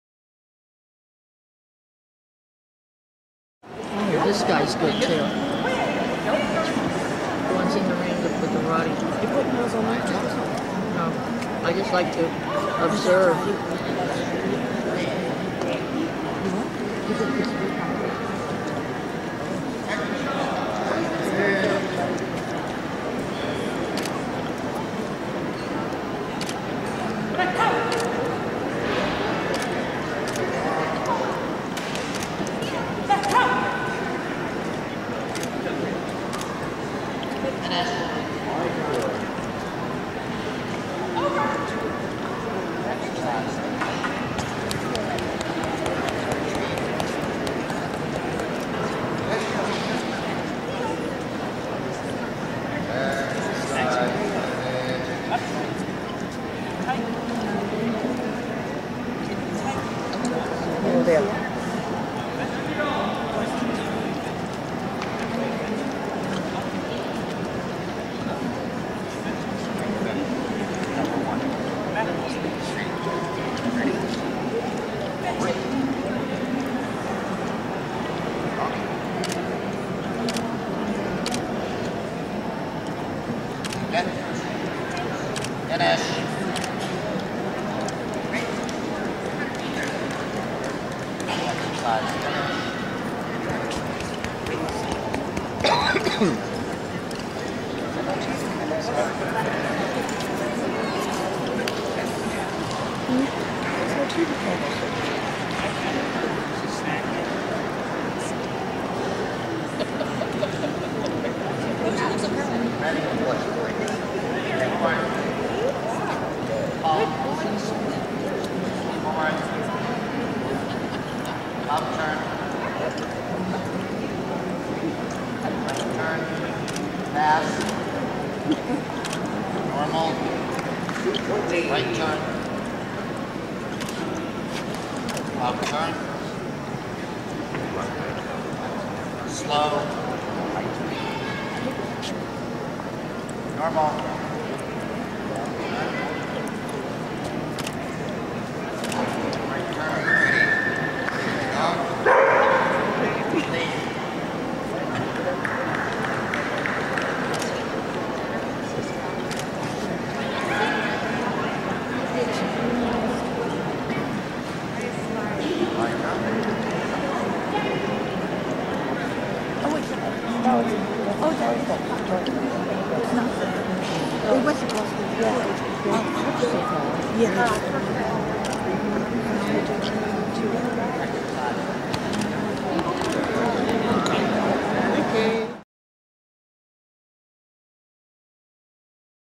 NOI 2011 RIng 2 Day 1
Mack competing in his first ring of the 2011 National Obedience Invitational in Orlando, FL. This was our first ring of the day - at roughly 4:30 a.m. our time/7:30 a.m. east coast time.